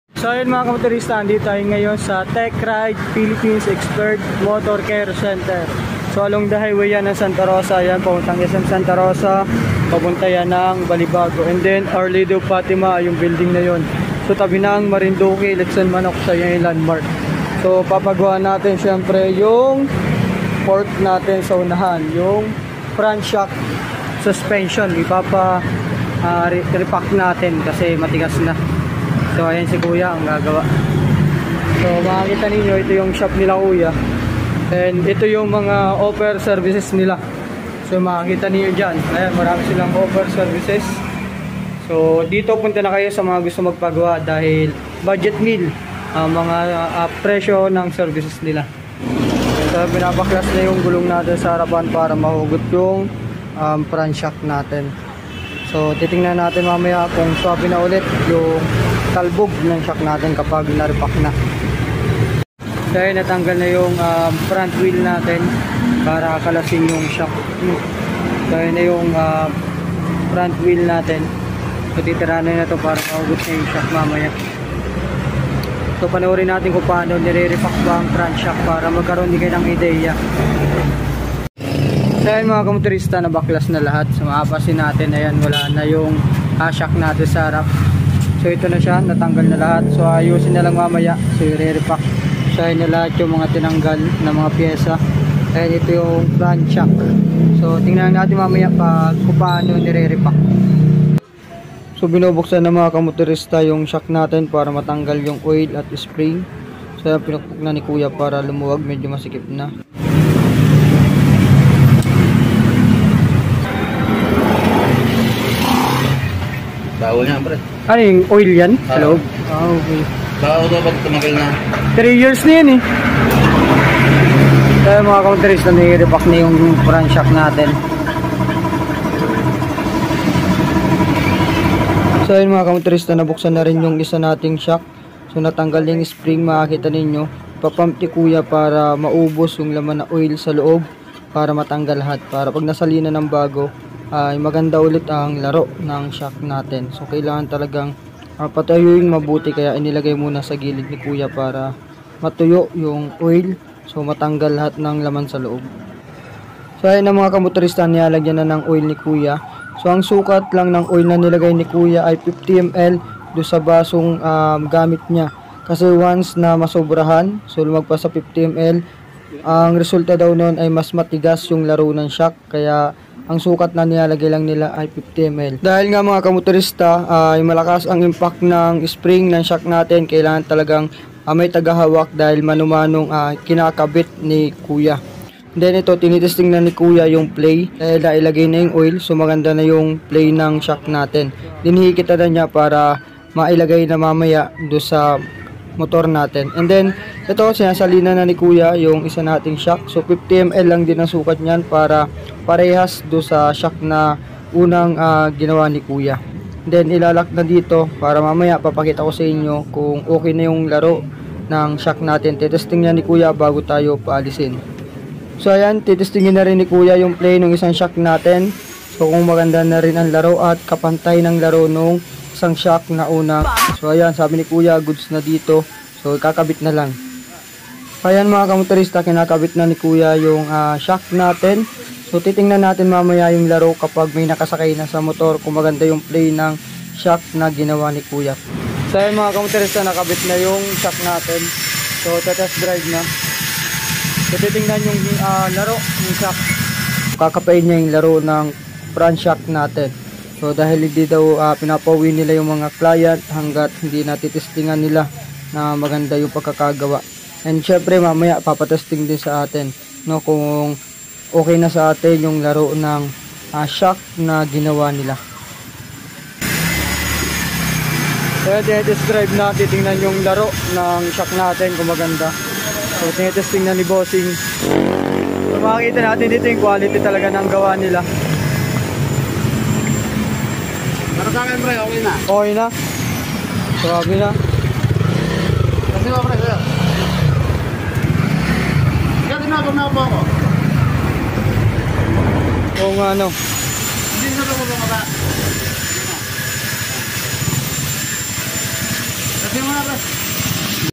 FRONT SHOCK REPACK BUDGET MEAL LANG
MURANG MANINGIL NA SHOP NA GUMAGAWA NG FRONT SHOCK REPACK at may magandang serbisyo ng paggagawa Techride Pilipinas